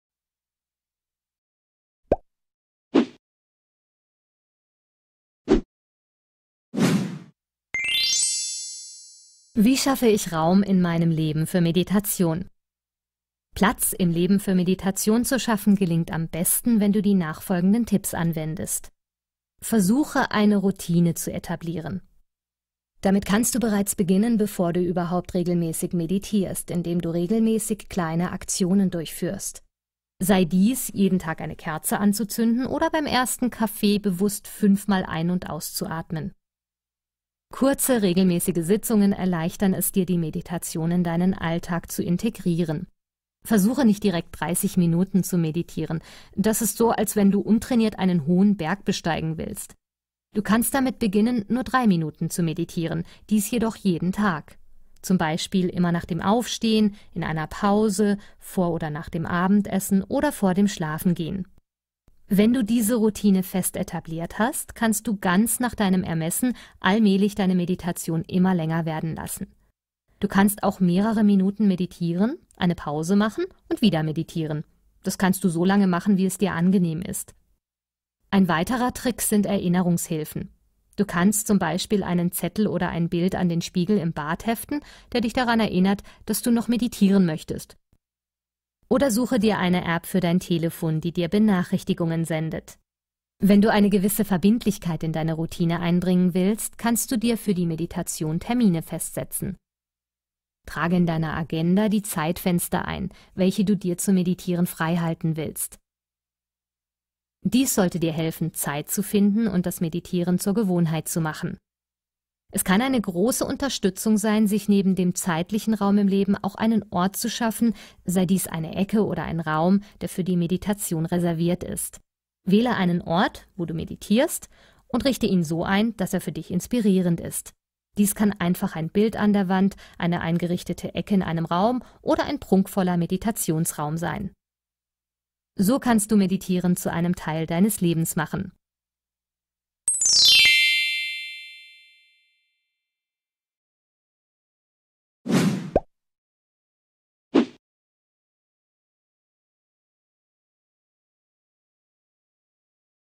5 Wie schaffe ich Zeit und Raum
Meditieren lernen, eine Einführung in die Meditation in 7 Videos.

Es scheint manchmal schwierig im alltäglichen Leben Raum zu schaffen, um zu meditieren...
Hier einige Tipps, wie dir dies besser gelingen kann

• Kurze, regelmässige Sitzungen
Du kannst damit beginnen nur 3 Minuten zu meditieren, dies jedoch jeden Tag. Nach und nach kannst du die Zeitspanne nach deinen Möglichkeiten verlängern.
• Erinnerungshilfen
Du kannst zum Beispiel einen Zettel oder ein Bild an den Spiegel im Bad heften, der dich daran erinnert, dass du noch meditieren möchtest.
• Termine setzen
Trage in deiner Agenda die Zeitfenster ein, welche du dir freihalten willst zum Meditieren.
• Routine etablieren (Kaizan)

Dies sollte dir helfen Zeit zu finden und das Meditieren zur Gewohnheit zu machen.

an!